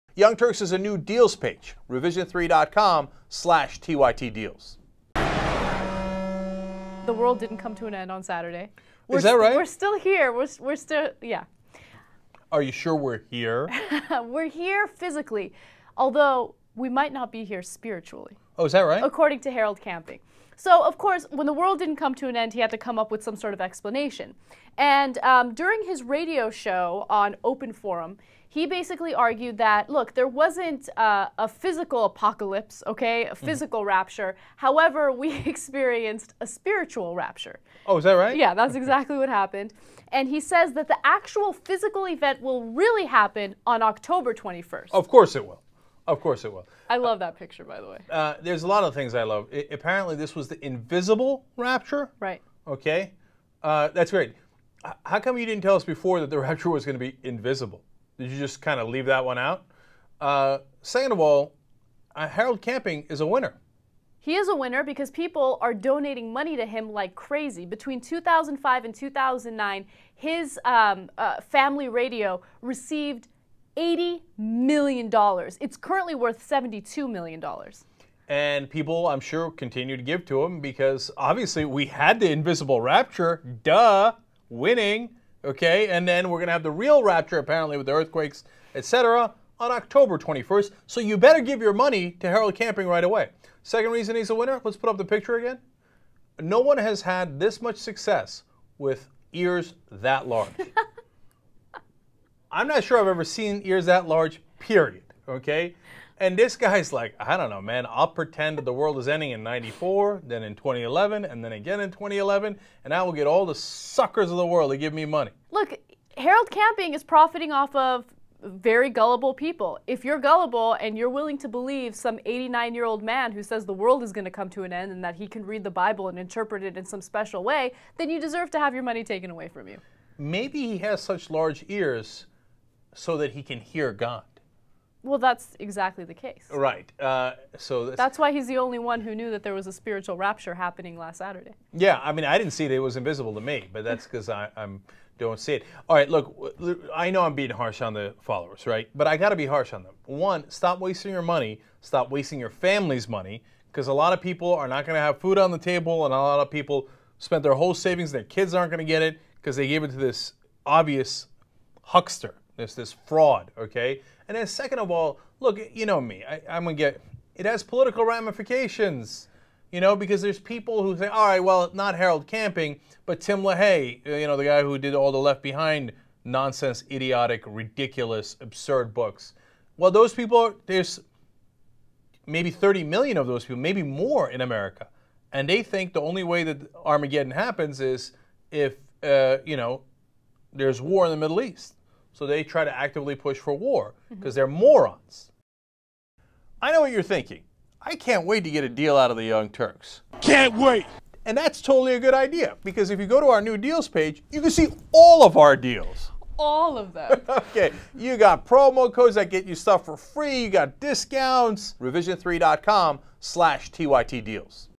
Harold Camping's New Rapture Date - October 21, 2011
Harold Camping explains when we'll experience the actual physical rapture. We've only experienced a spiritual rapture. The new End of the World date is October 21, 2011.